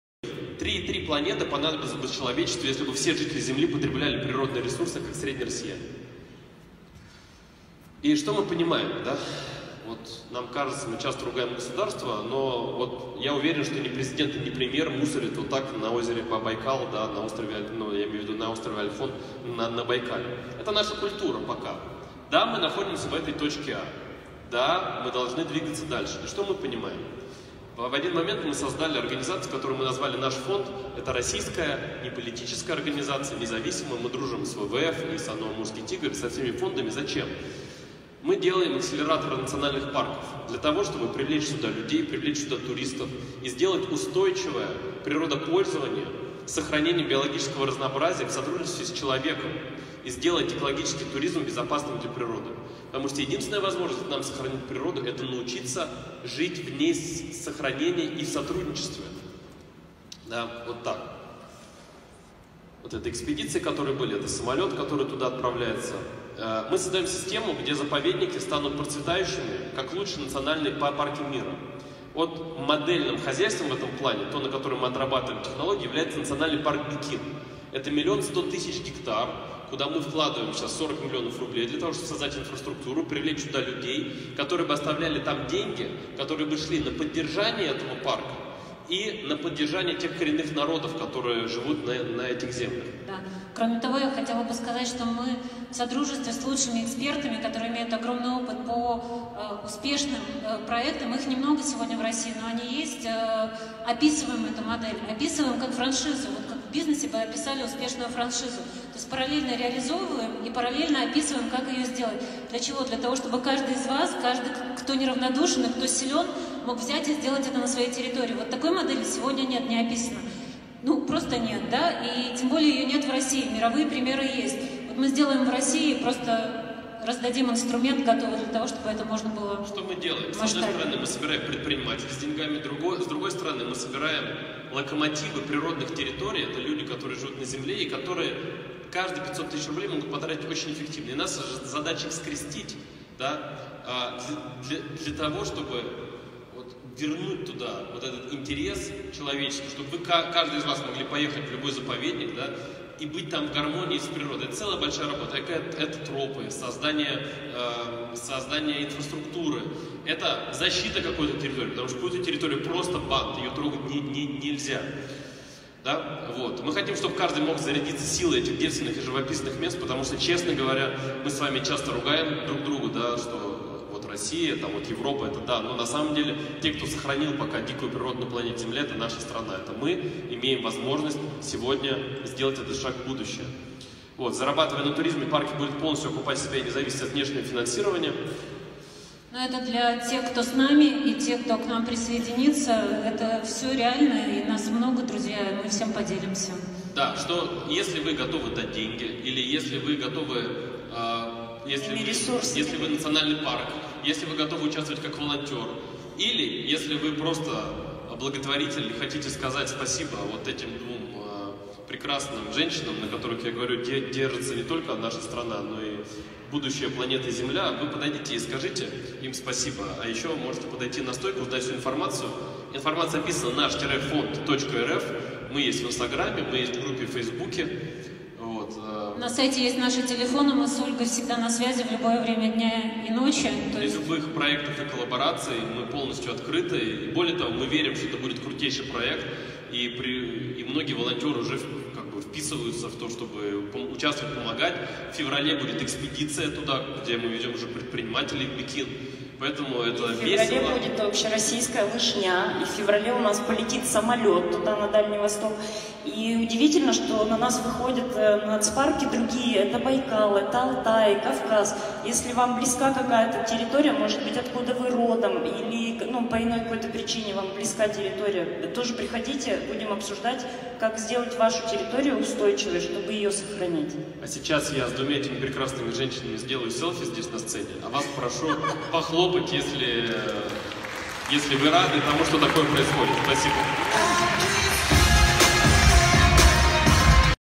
"Наш фонд" как франчайзер акселерации российских ООПТ. 13.12.2018
На посвящённом благотворительности форуме Synergy Charity Forum (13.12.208), организованном Московским финансово-промышленным университетом “Синергия”, соучредители (август 2018 г.) Фонда поддержки и развития природоохранных и других социально-значимых проектов “Наш фонд” О.И.Лазарева и О.М.Бандалова вместе с основателем проекта “Бизнес молодость” П.В.Осиповым декларируют направления работы фонда с особо охраняемыми природными территориями.

Источник видео – Фейсбук-аккаунт директора Фонда “Наш фонд” О.М.Бандаловой: Ольга Бандалова (Фейсбук). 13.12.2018. URL